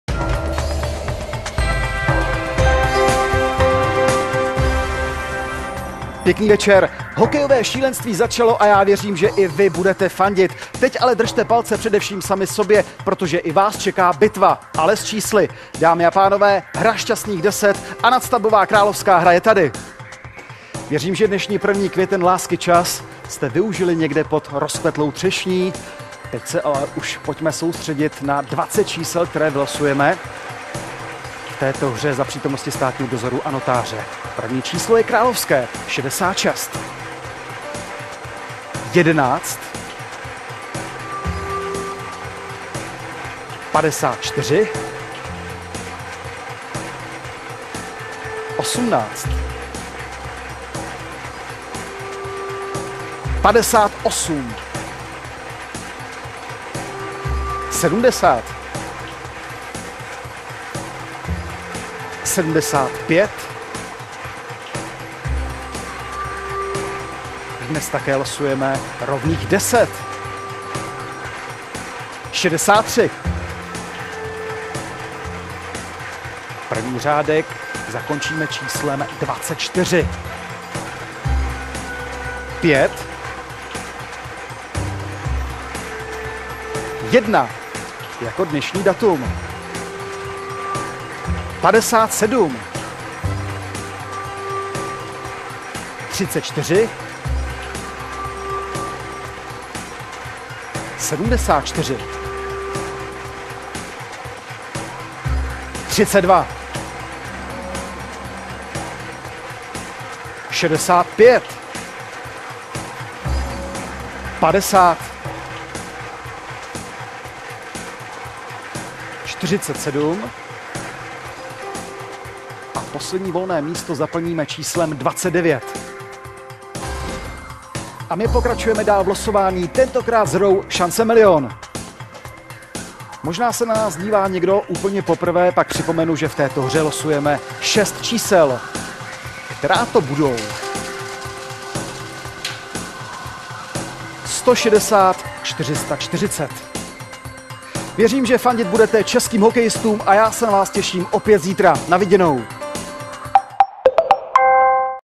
Š10 - 1. 5. 2015
Podívejte se na záznam slosování Š10 a doplňkové hry Šance milion.